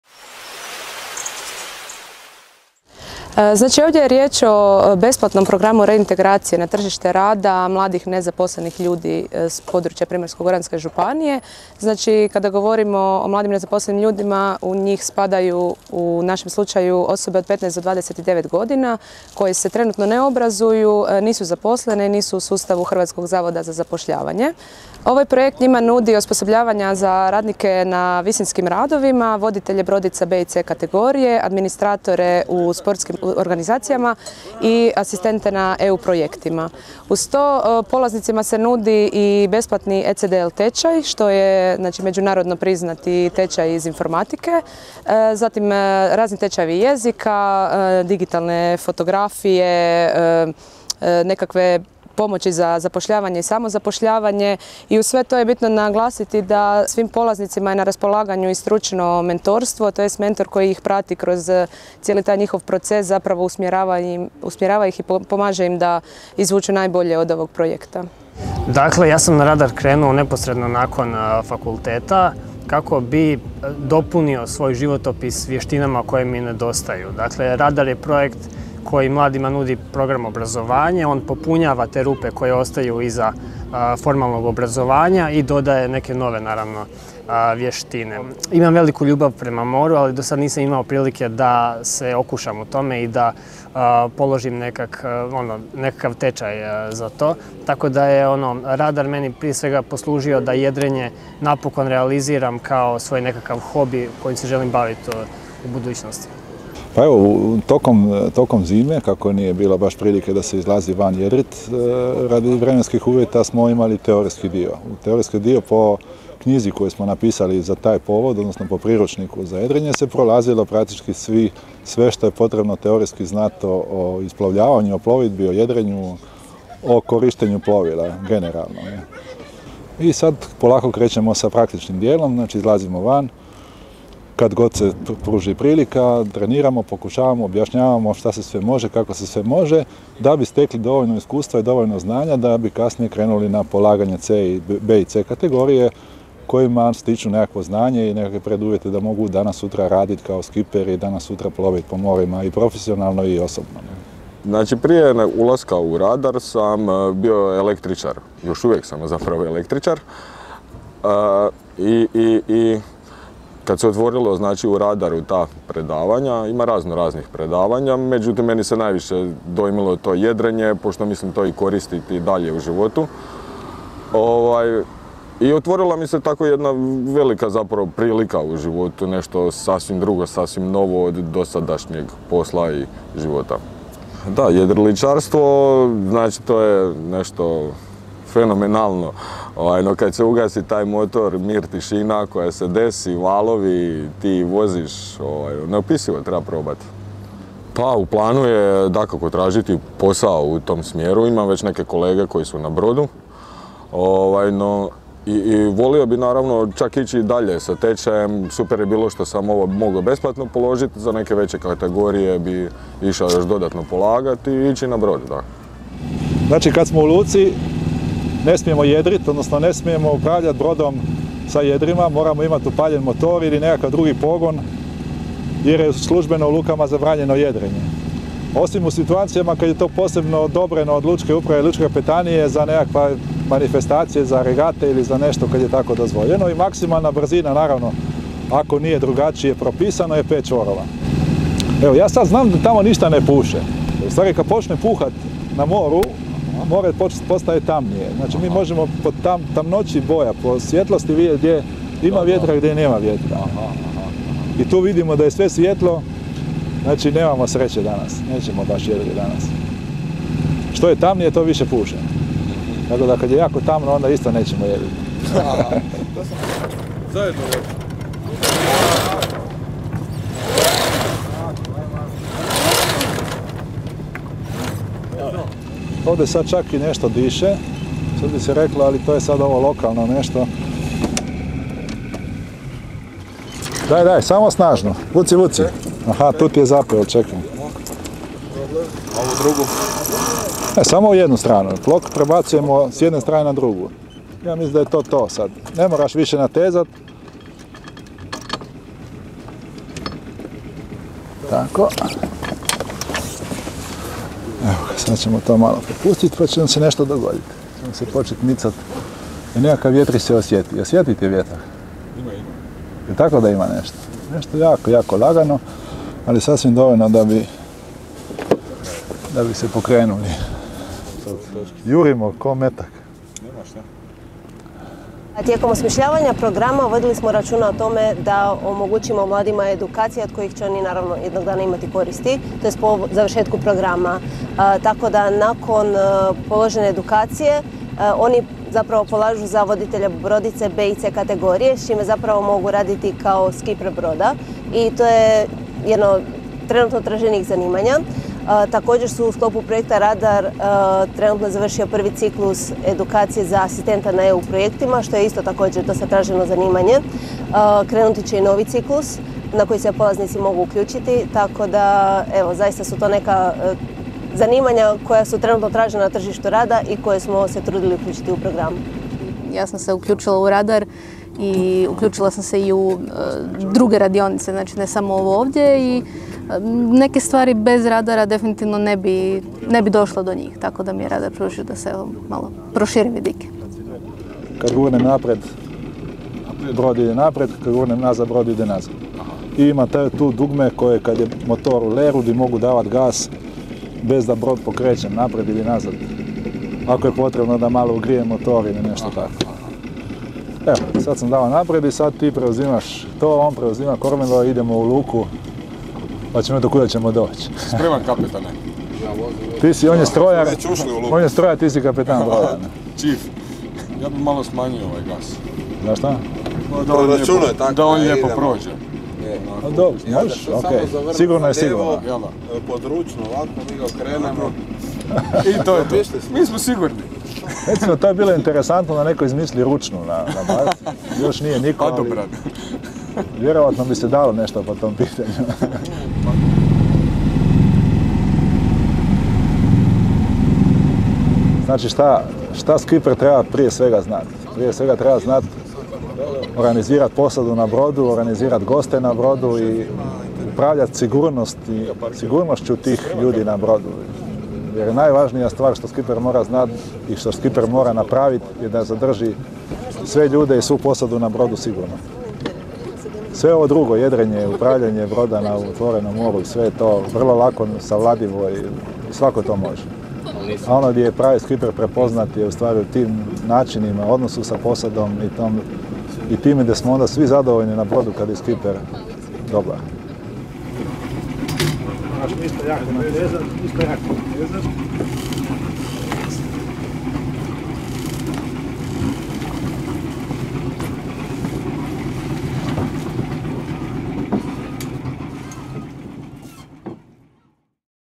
MojaRijeka.hr - Projekt Radar za mlade Centra tehničke kulture Rijeka
Proveli smo dan s budućim voditeljima brodica - polaznicima besplatnog programa reintegracije neaktivnih mladih osoba na tržište rada s područja Primorsko-goranske županije, a što ga provodi Centar tehničke kulture Rijeka.

Riječ je o projektu Radar koji mladima od 15 do 29 godina, koji se trenutno ne obrazuju, nisu zaposleni i nisu u sustavu Hrvatskog zavoda za zapošljavanje, nudi nekoliko vrsta osposobljavanja - za radnike na visinskim radovima, voditelje brodica B i C kategorije, administratore u sportskim organizacijama i asistente na EU projektima.

Nakon odrađenog teorijskog dijela s mentorima Jedriličarskog kluba Maxi, radarovcima je došlo vrijeme za izlazak na more i upoznavanje s jedrilicom i jedrenjem, a kako im ide, jesu li se zaljubili u jedrenje i što dalje planiraju, saznajte u našem videu!

Autorica reportaže je Sanja Kapidžić.